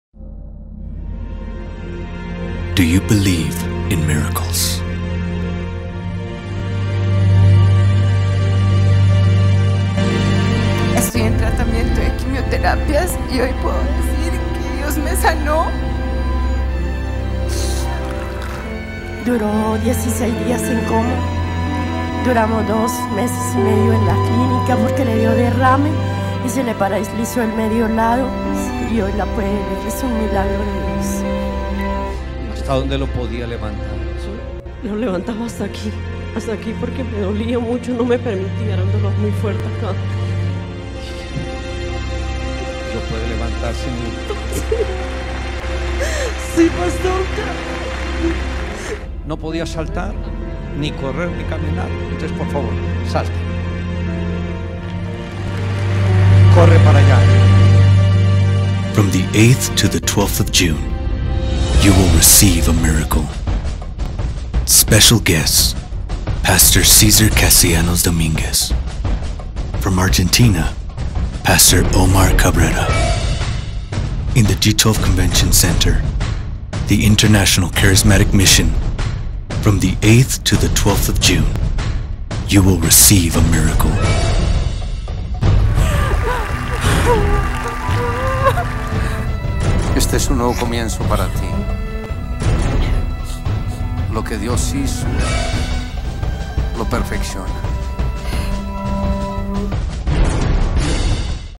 Week Miracles
Because of the power of the blood of Jesus, our lives will not be the same, as his presence will bring a miracle to our lives. From June 8th to the 12th at 6:30 PM at the G12 Convention Center in Bogota, Colombia, you will experience supernatural miracles. Come and be part of a new move. Host pastors César and Claudia Castellanos, and as a special guest, from Argentina, Omar Cabrera will be speaking during this week.